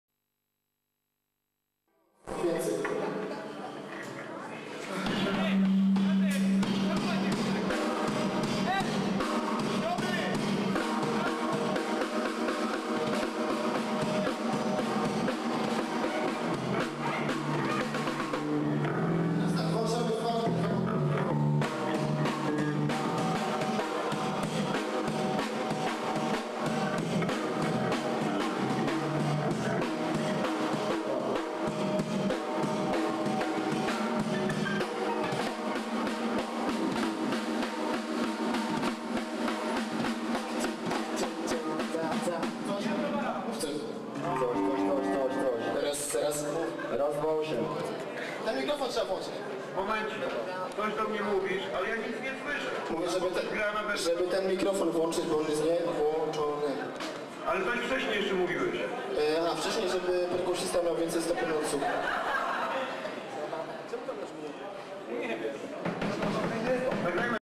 moutza + NTK - fragmenty próby przed koncertem
wystepuja: ksantyp liper novy tek seba victor savik i inni
jeden z ostatnich wystep[ow zespolu moutza
proba przed koncertem wspolie z chlopakami z NTK